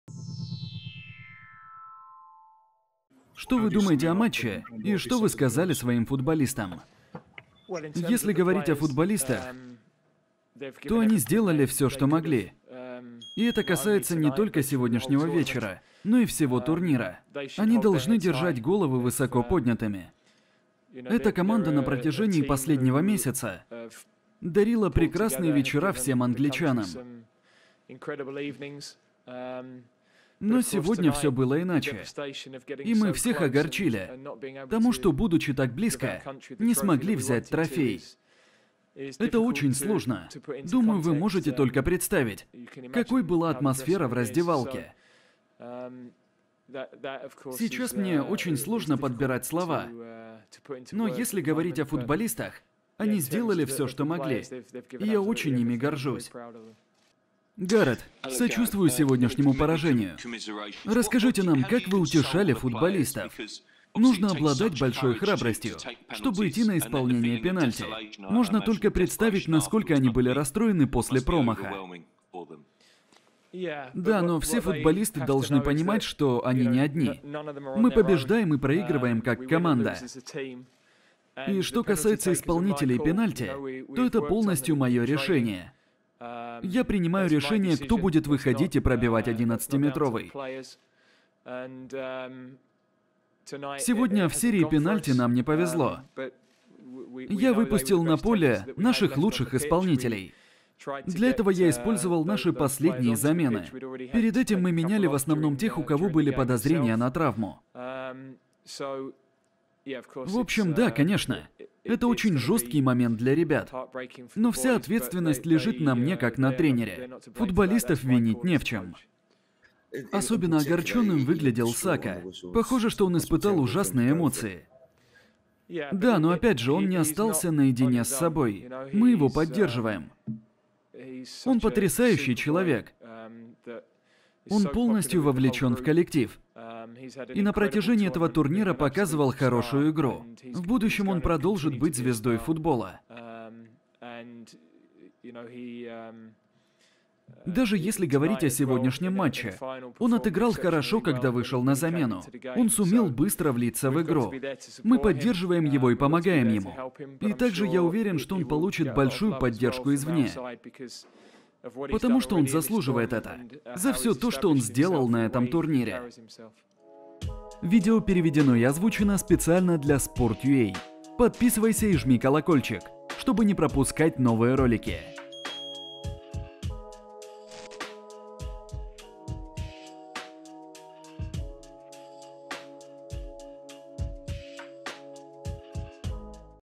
САУТГЕЙТ про СЕРИЮ ПЕНАЛЬТИ и итоги ЕВРО-2020 | Италия - Англия
В финале Евро-2020 на лондонском стадионе "Уэмбли" сборная Италии обыграла команду Англии со счетом 3:2. Судьба трофея Евро решилась в серии пенальти.

Уже на второй минуте встречи мяч после подачи от правого фланга к левой стойке отправил в ворота итальянского вратаря Джанлуиджи Доннаруммы защитник сборной Англии Люк Шоу. Как отметили комментаторы, Шоу стал автором самого быстрого гола в финале в истории чемпионатов Европы. Он отличился на временной отметке в 1 минуту и 56 секунд.

Во втором тайме итальянцам удалось сравнять счет. На 67-й минуте центральный защитник Италии Леонардо Бонуччи уложил мяч в ворота соперников после углового. Как напомнили в УЕФА, Бонуччи (номер 19) - самый возрастной (34 года и 71 день) автор гола в истории ЧЕ.

Затем финал Евро-2020 перешел в овертайм. До этого по аналогичному сценарию проходил матч Англия - Дания (2:1). Первый тайм в дополнительное время завершился без изменений в счете. Второй экстра-тайм картины не изменил. Впервые за 45 лет судьба трофея Евро решалась в серии пенальти.

Вашему вниманию запись пресс-конференции Гпрета Саутгейта после финального матча ЕВРО-2020 Италия - Англия.